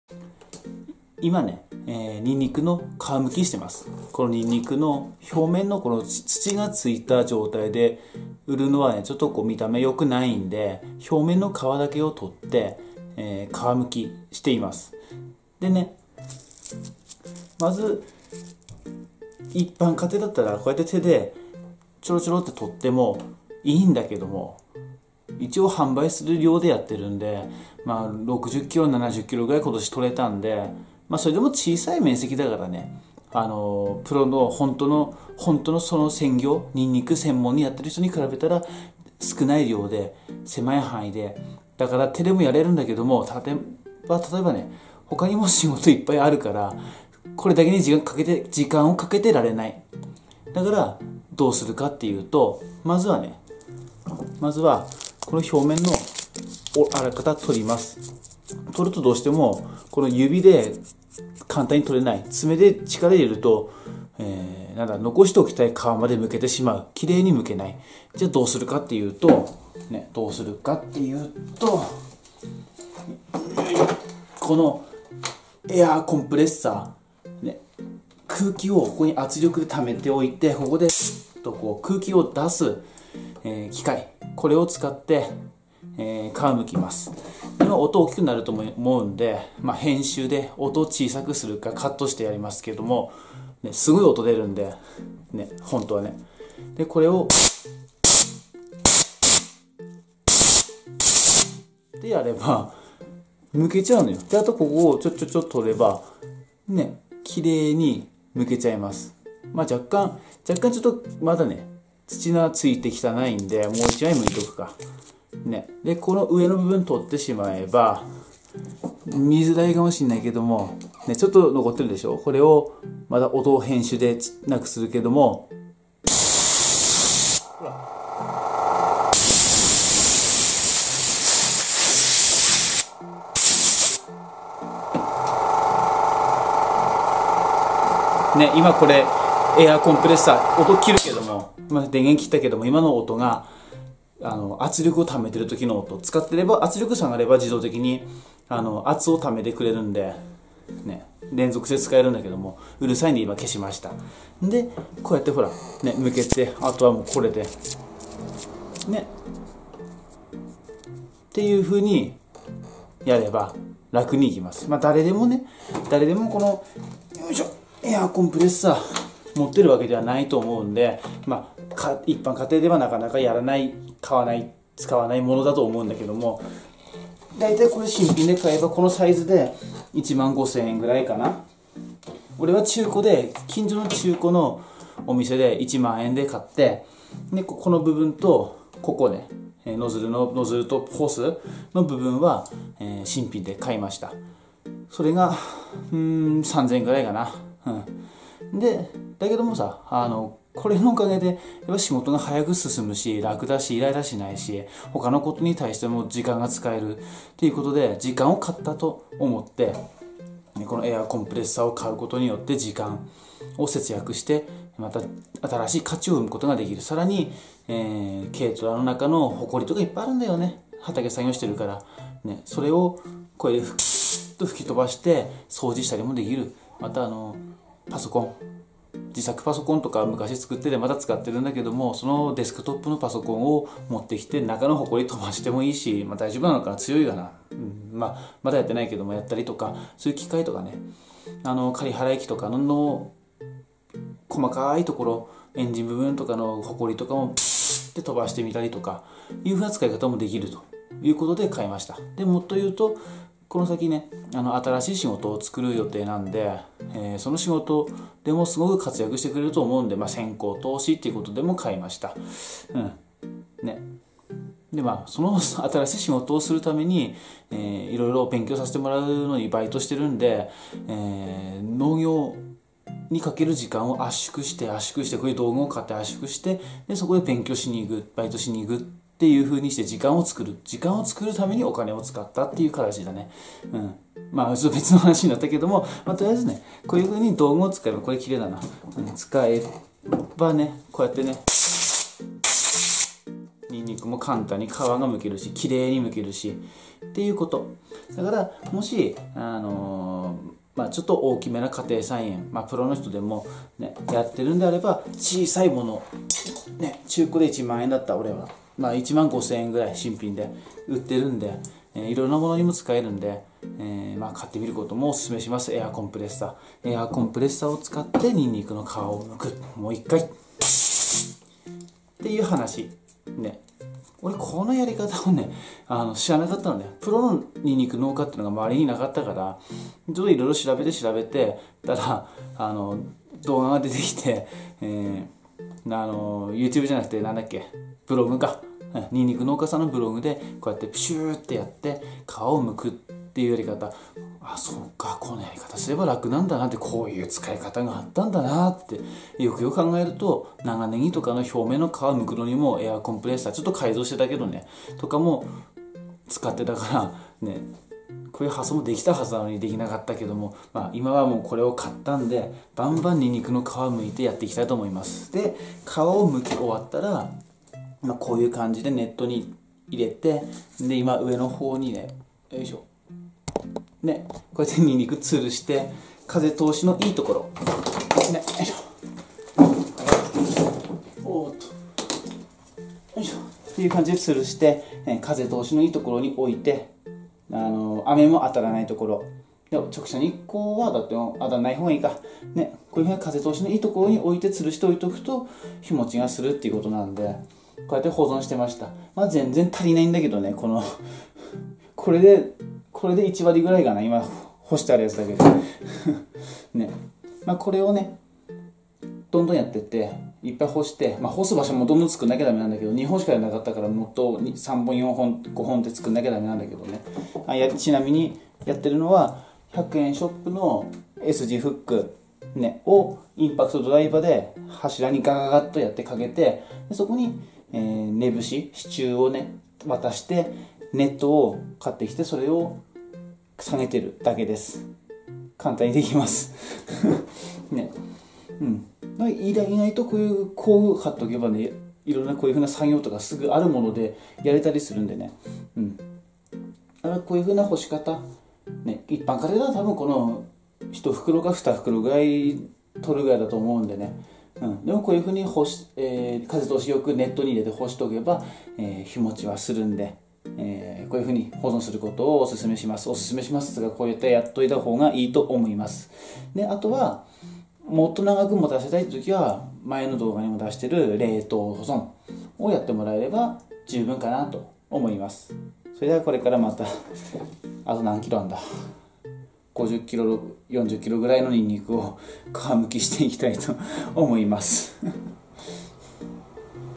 ニンニクの皮むきと保存方法【エアーコンプレッサー】
前から欲しかったエアーコンプレッサー買ったら作業効率が上がって、ほかの仕事の時間が作れました。